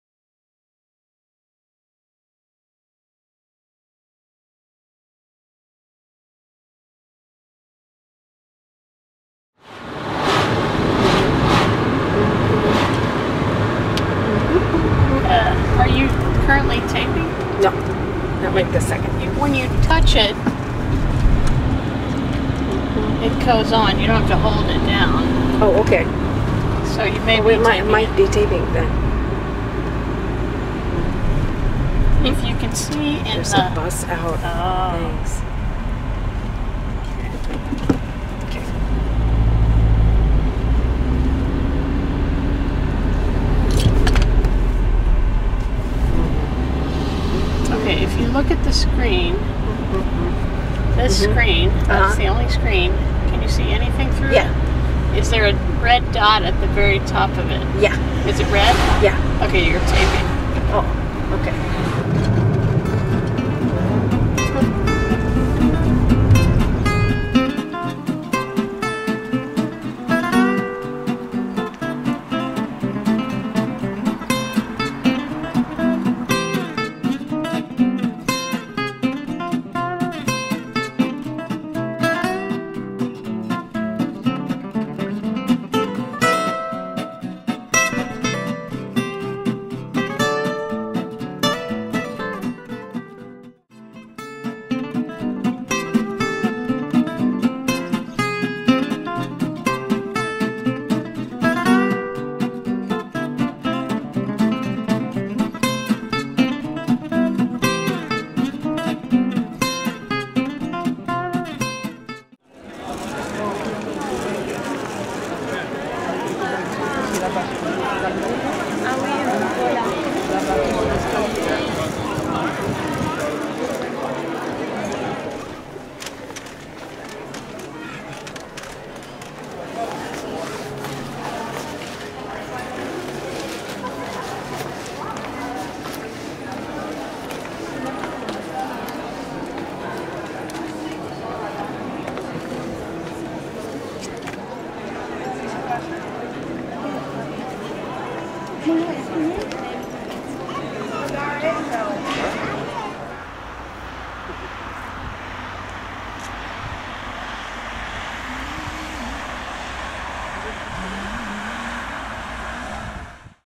A Day in Tuscany
Out and about in a rental car in the Chianti region of Tuscany. Includes a visit to San Gimignano, a medieval hill town.